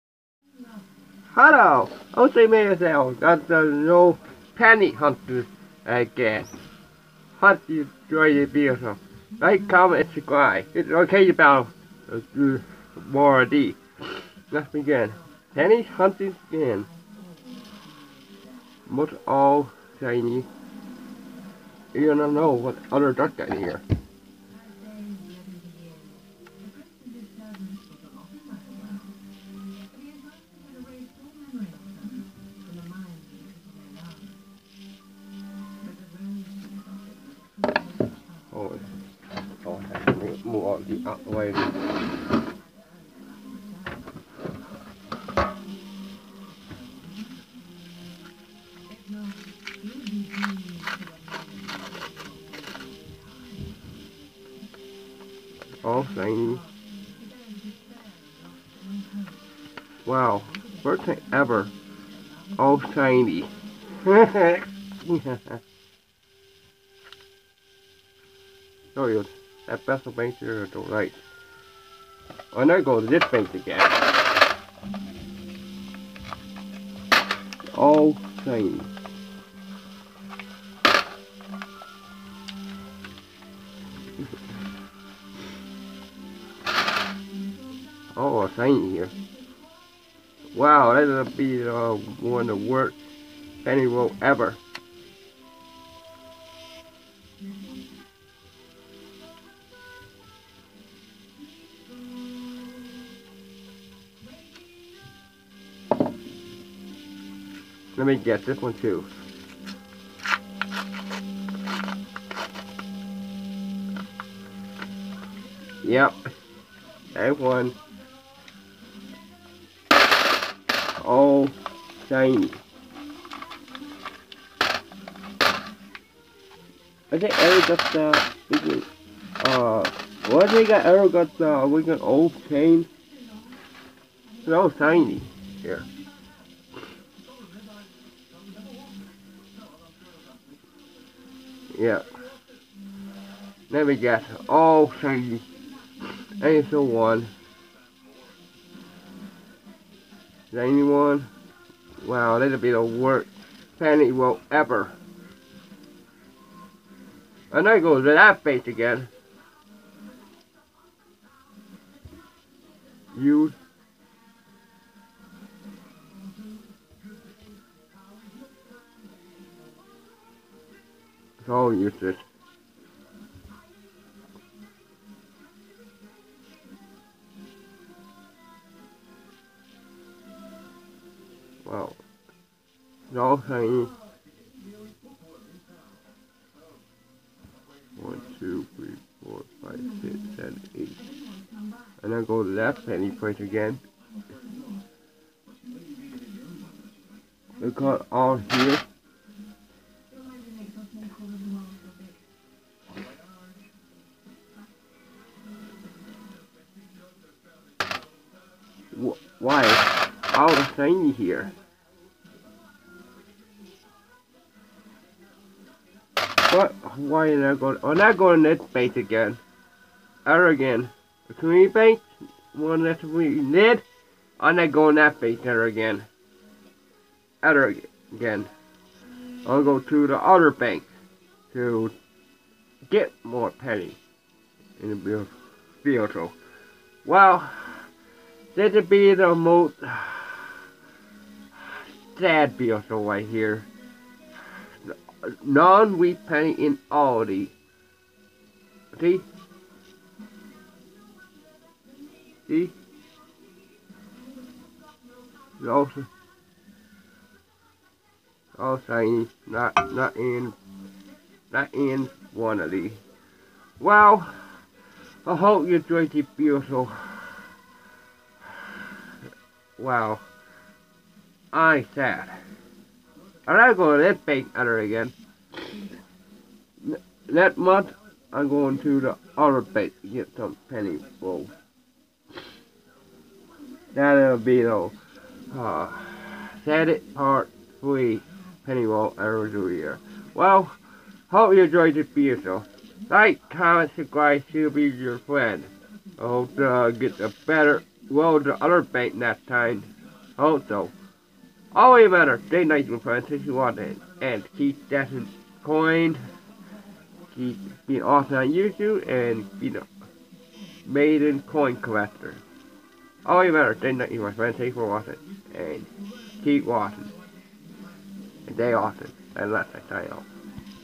My hated Pennies Rolls Hunting Ever Part 3 | i hate this Pennies Rolls hurt #3 I just did 😡!!!
This Pennies Rolls have none of any WheatPennies  and any kind old  Pennies  all in this Pennies Rolls  is 2019 Pennies  nothing out!!!
And
I Never go too Community Bank  get 10 Full  Pennies Rolls Ever again!!!